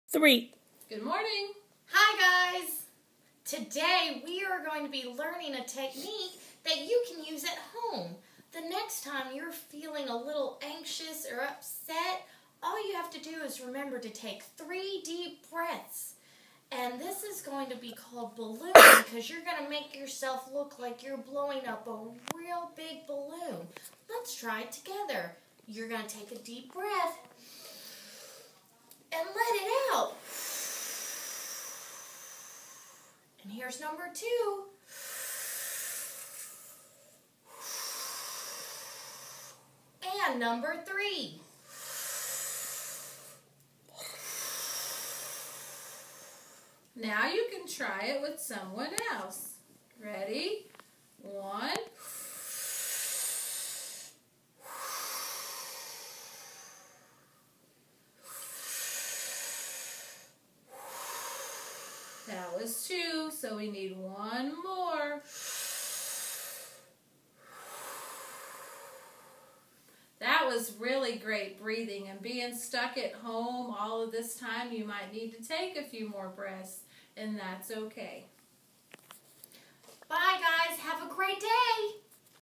CADC Malvern Early Head Start - Mrs. Santana & Ms. Katie - Balloon Breathing
Mrs. Santana & Ms. Katie are using baby dolls to help with balloon breathing technique